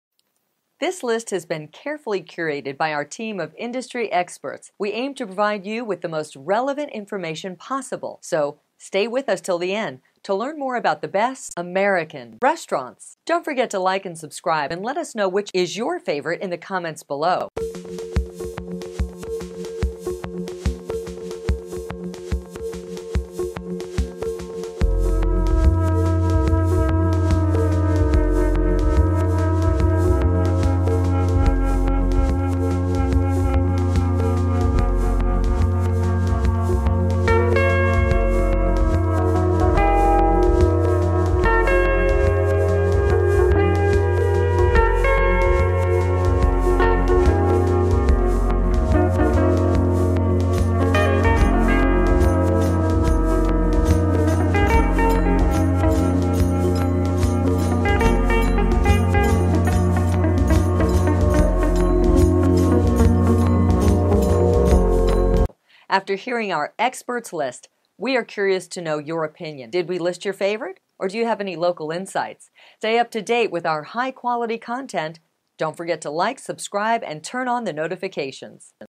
AMERICAN RESTAURANTS in Cummaquid, MASSACHUSETTS
This is our list of the 5 best places in Cummaquid.
If you want to increase your own cooking abilities, be sure to check out Jamie Olivers latest Cookbook:
5 Ingredients: Quick & Easy Food on amazon*

These are our Top 5 places for Cummaquid:
 00:17 - Intro
 00:47 - Our Top 5
 00:13 - Outro

The given order of the places doesn´t represent a ranking, it´s more a list of our top 5 recommendations for Cummaquid.

637f94a28f047373ee4e6f3b